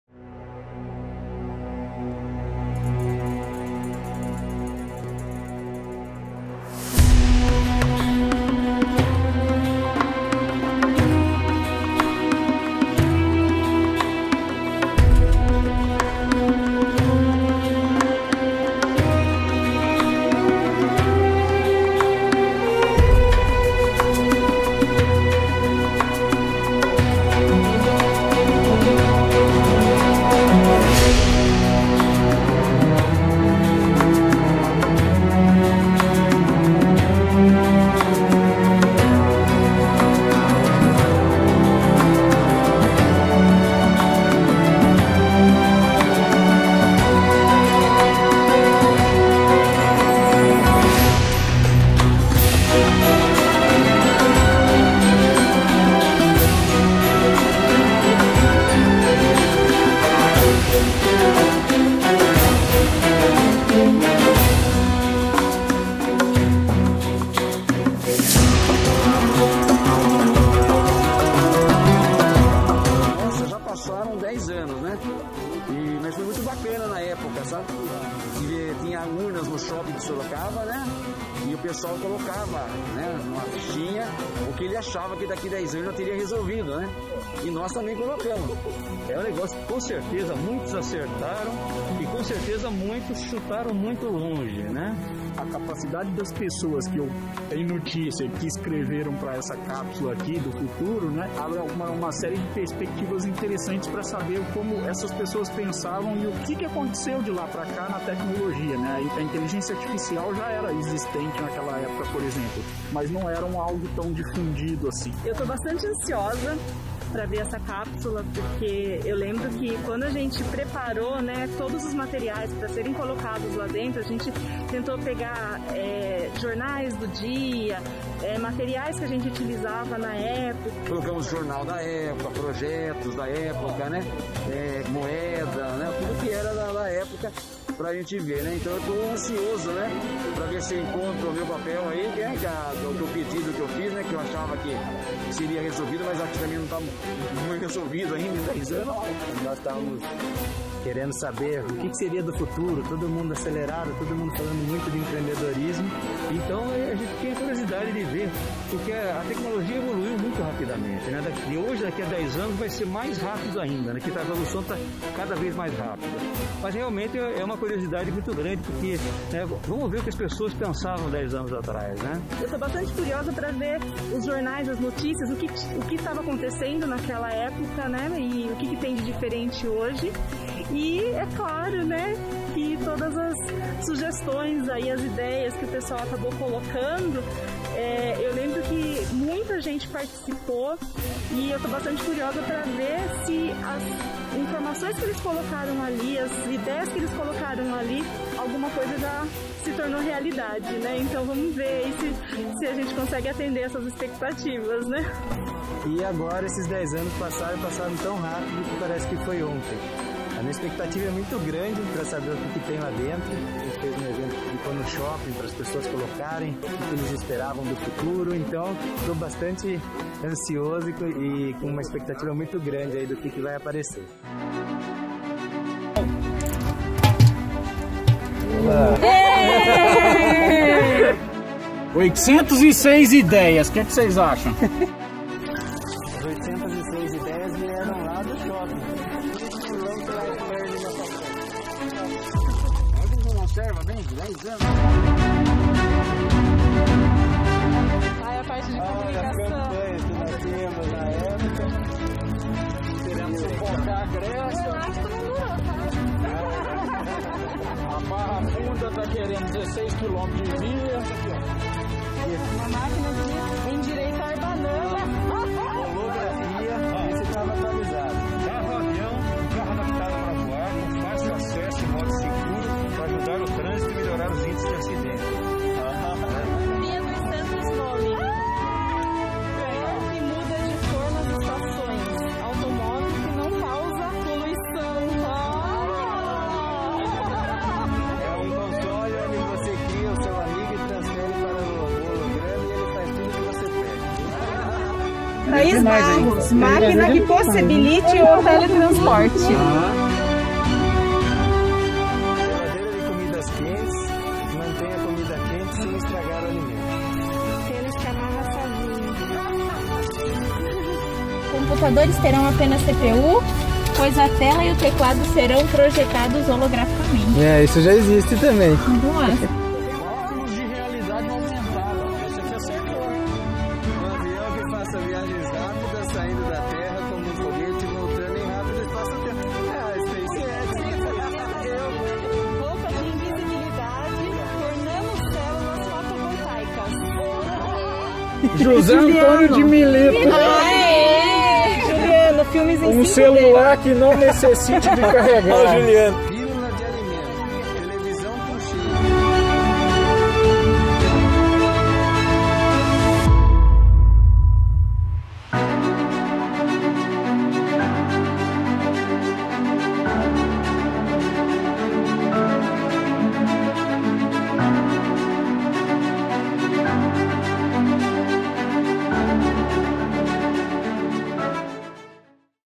45 anos Facens: Abertura da Cápsula do Tempo
Em 2011, a Facens celebrou os seus 35 anos. Uma das ações para marcar a data, foi a criação de uma cápsula do tempo, que seria aberta 10 anos depois. Alunos, professores, colaboradores e toda comunidade Facens foi convidada a escrever uma mensagem para o futuro, que vieram para a cápsula e foram todas enterradas.

10 anos se passara e, em comemoração aos 45 anos de Facens, a cápsula foi aberta!
Para acompanhar esse momento tão aguardado, algumas pessoas que estavam na época que a cápsula foi enterrada foram até a Facens para desenterrá-la e ver se os sonhos e desejos para a data se concretizaram!

Acompanhe essa aventura com a gente!